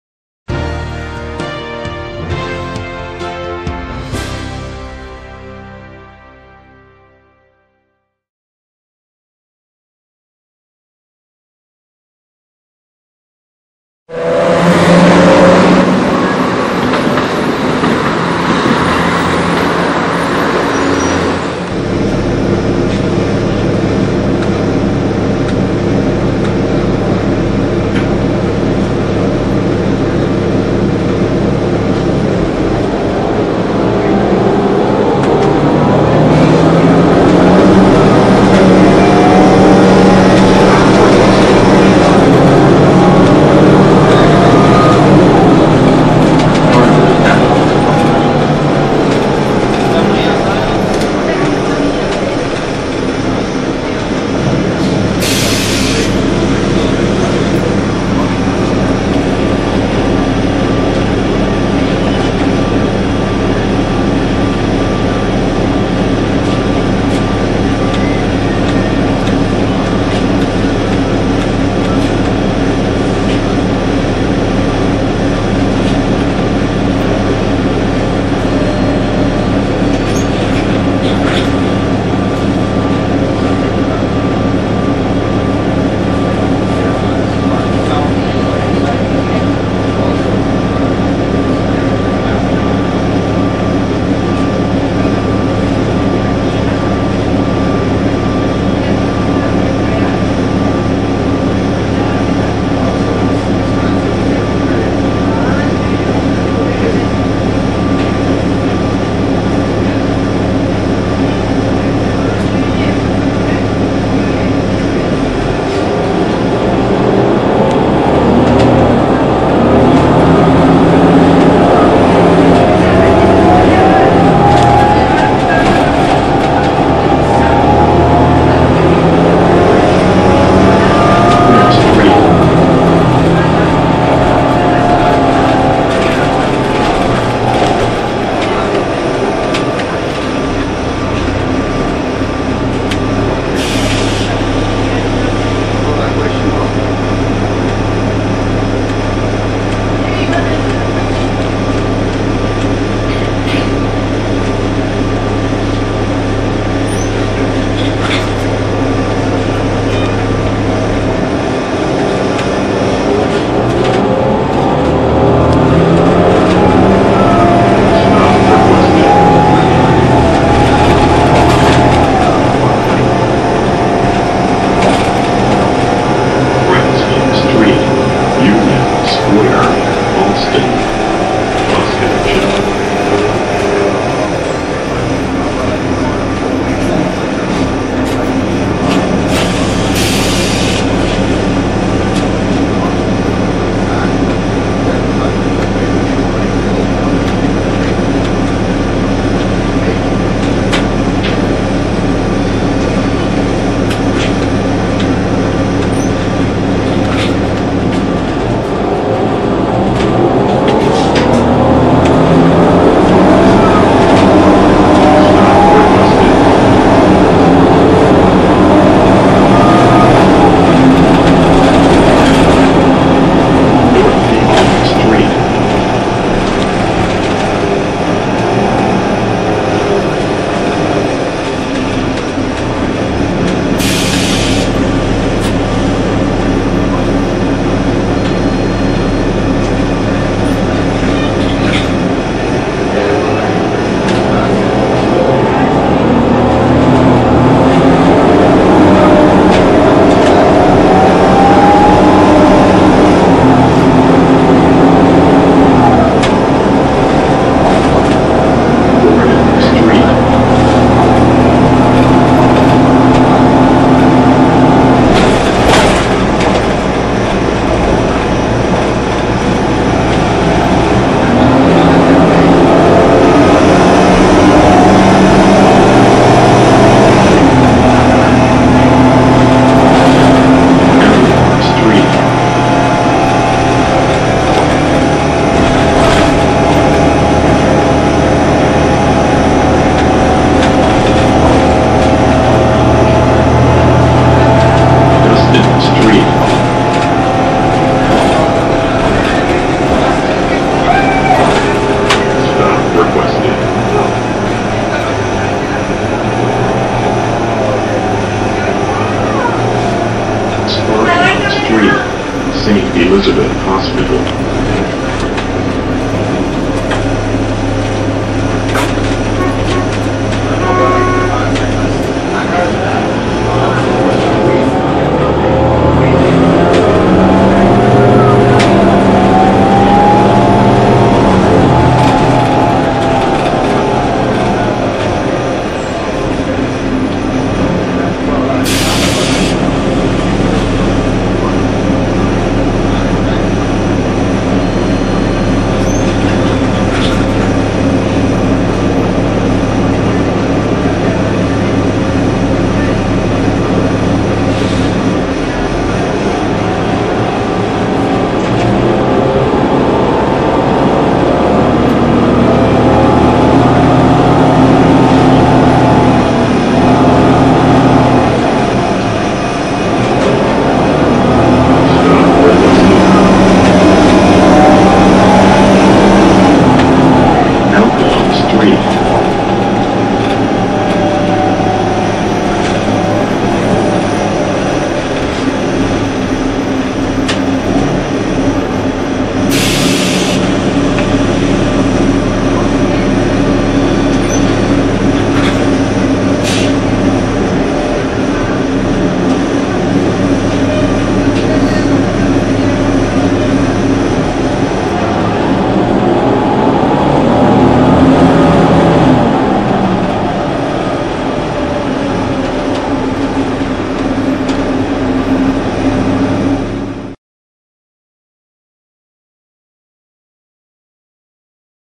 Massachusetts Bay Transportation Authority 2003 Neoplan AN440LF #0453
Welcome aboard MBTA Neoplan AN440LF #0453 as it operates on Route 57 from Kenmore Station to Watertown Yard via Brighton Avenue. 0453 is one of 193 purchased by MBTA in 2004 and has the distinction of being among the last batch of Neoplan Low Floors built in North America.